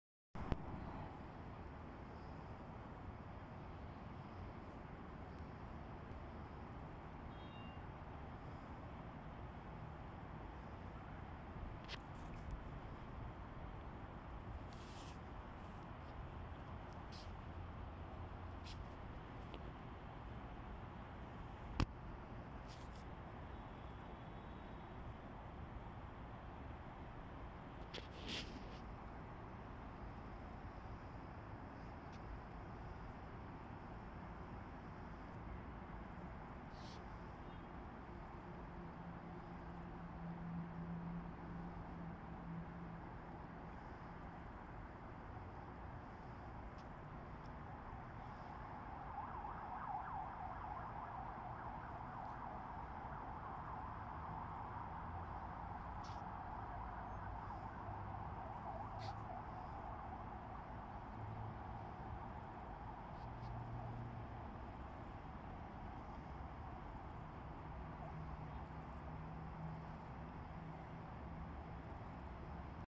suraj Per ye kya hai
Riyadh mobile camre se liya gya video.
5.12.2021